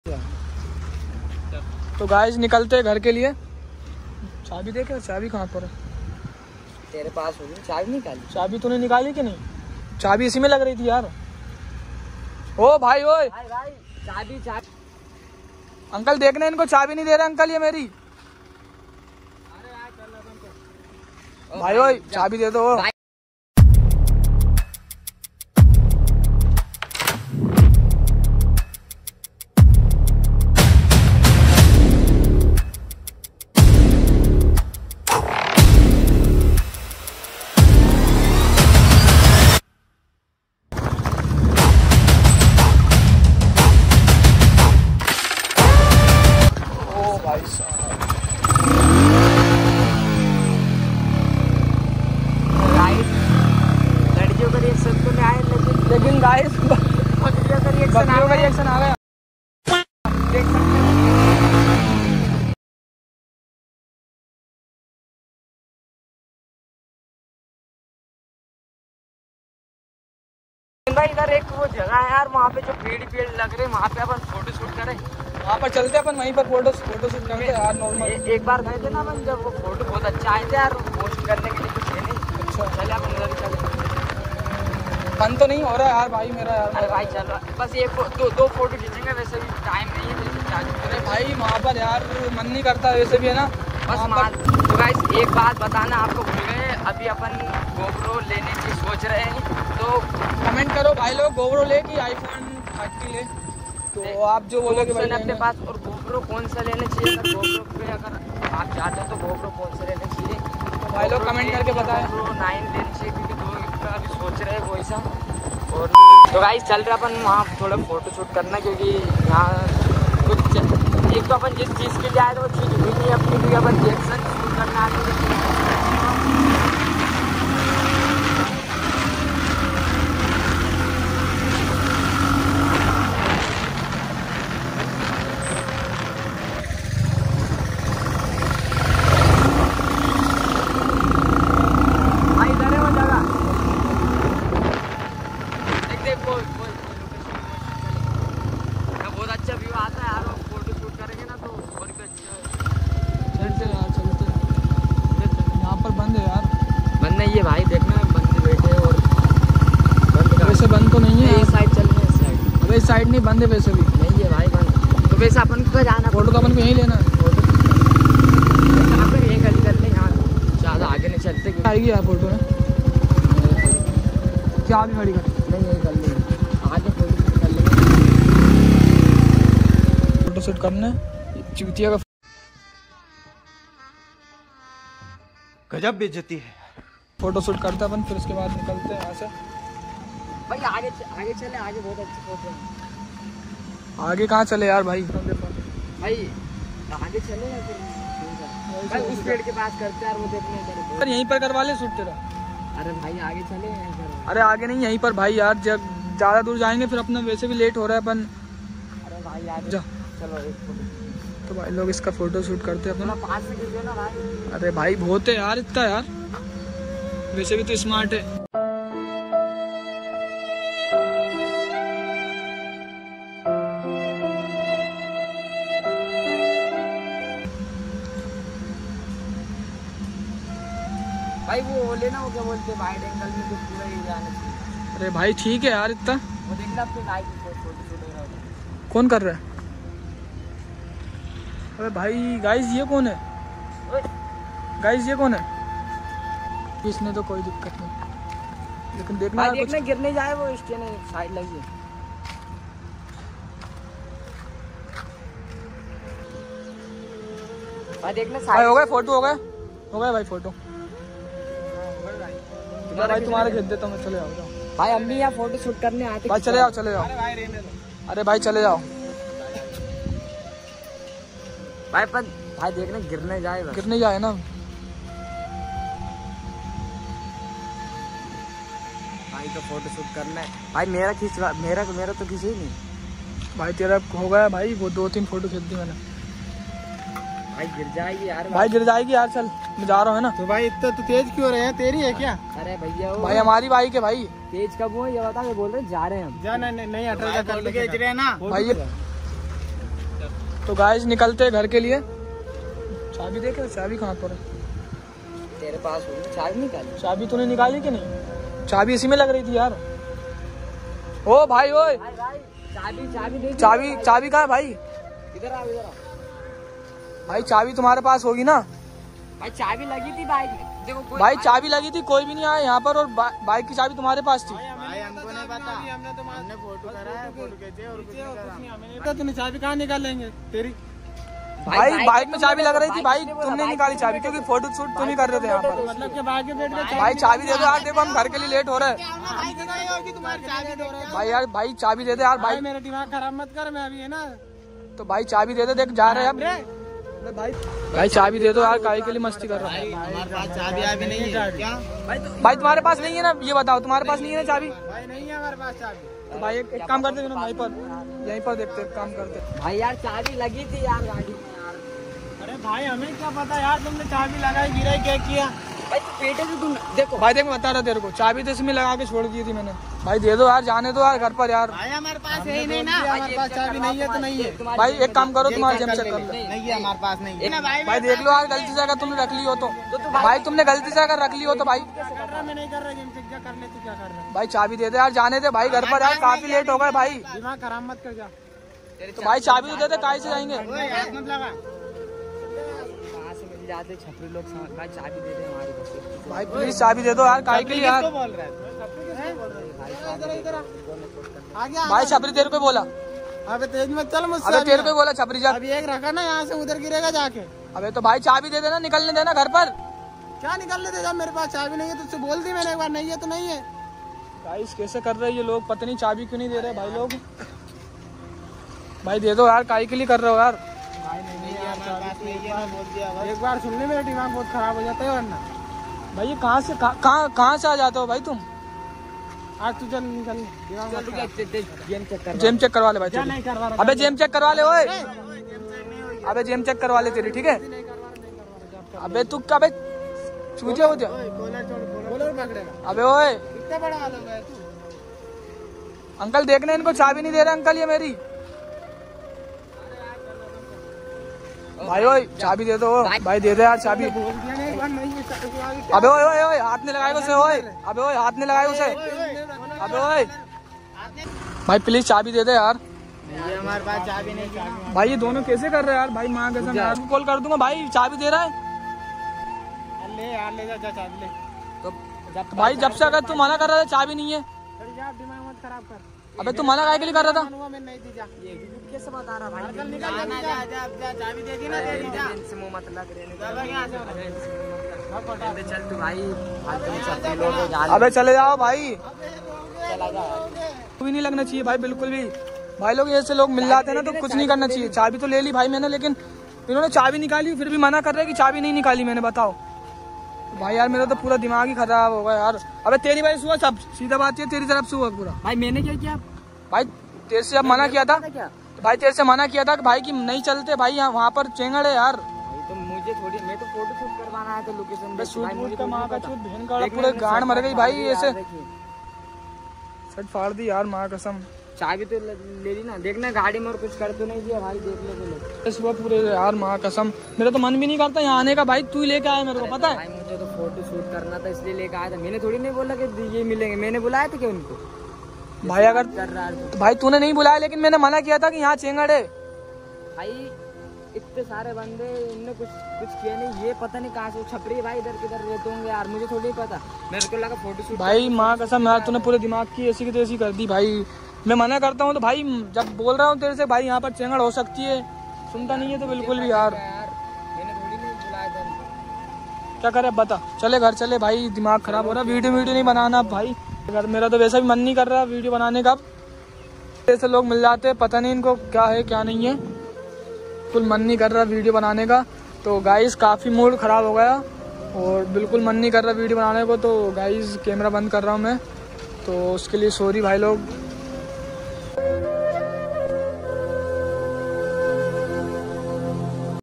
⚠️@THEMP04RIDER3.O Ke chakkar mei ho gai ladai 😡 Chhapri ne bike ki chabi chori kar li chakkar mei
@THEMP04RIDER  Ke chakkar mei ho gai   ladai 😡 Chhapri ne bike ki chabi chori kar li chakkar mei